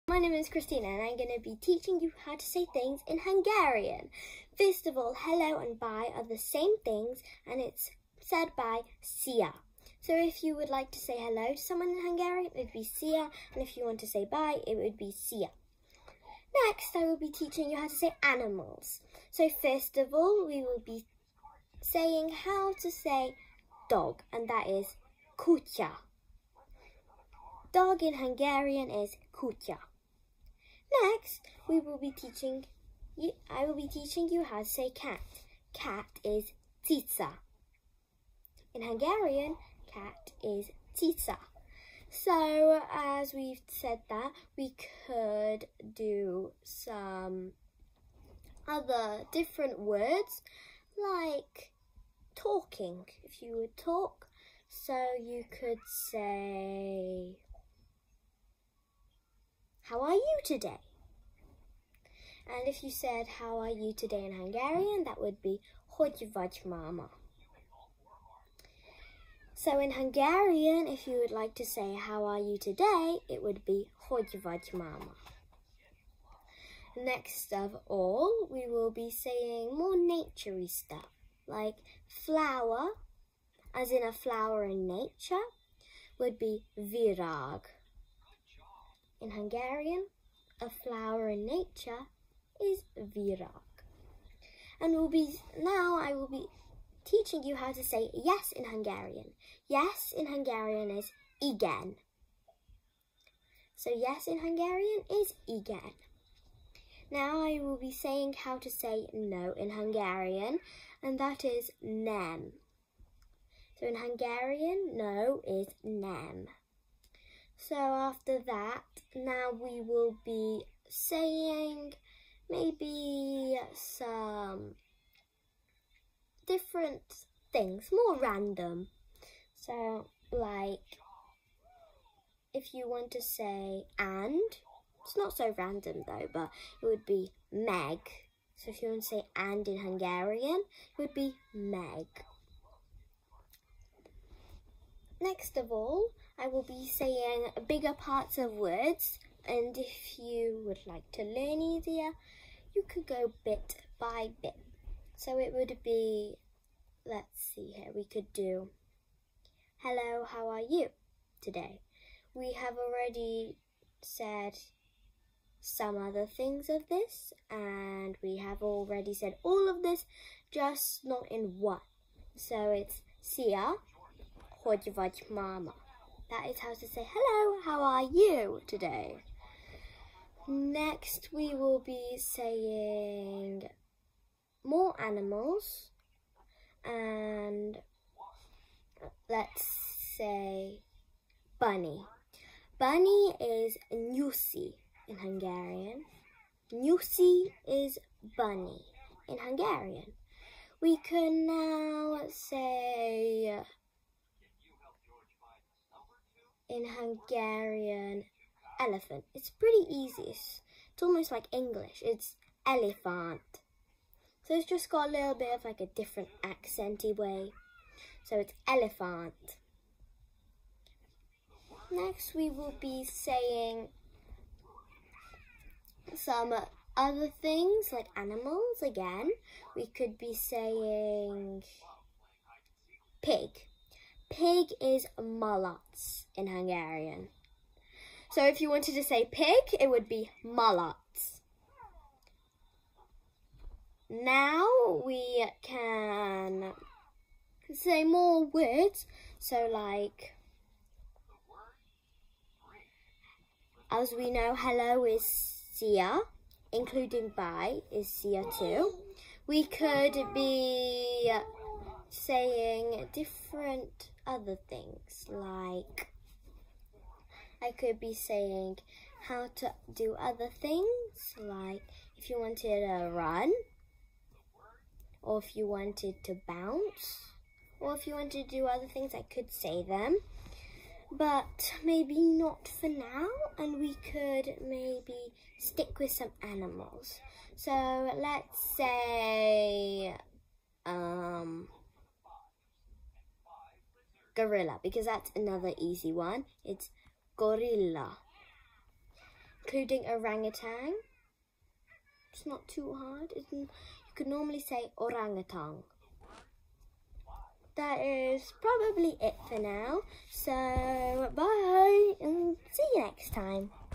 Kristina talking Hungarian!
This video can teach you how to speak Hungarian! 😁